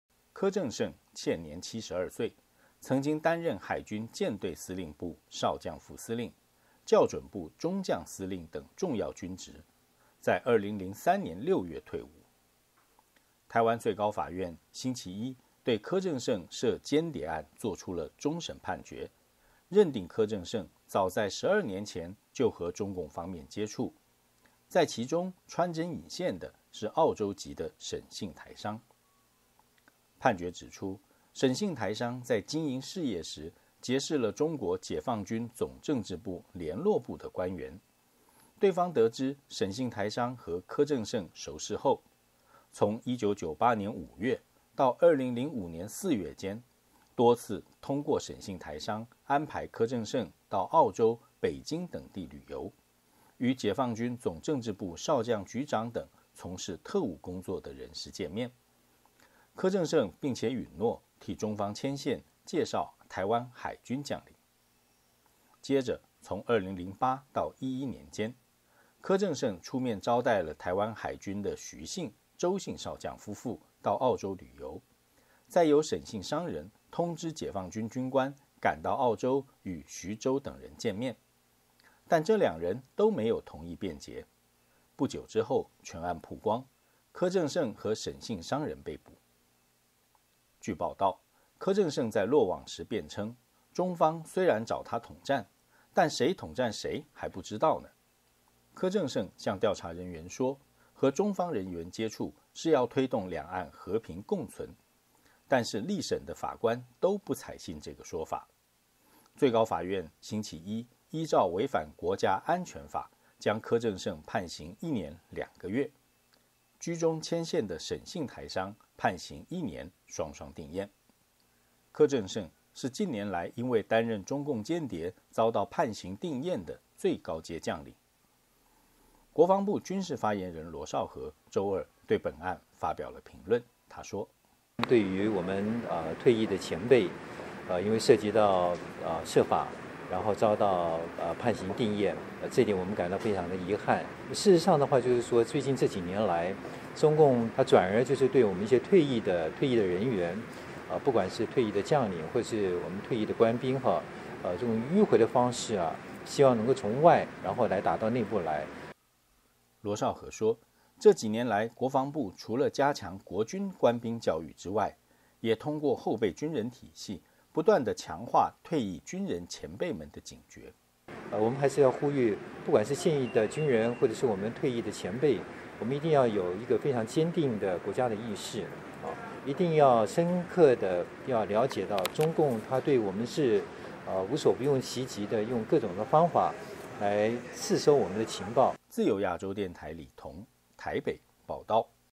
台最高法对柯政盛涉间谍案做终审判决
国军再传间谍案。海军退役中将柯政盛周一被台湾法院依间谍罪判刑确定。柯政盛也是近年涉中共间谍案军阶最高的退役军官。

柯政盛现年七十二岁，曾经担任海军舰队司令部少将副司令、教准部中将司令等重要军职，二零零三年六月退伍。

台湾最高法院周一对柯政盛涉间谍案做出终审判决。认定柯政盛早在十二年前就和中共方面接触。在其中穿针引线的是澳洲籍的沉姓台商。

听众谈话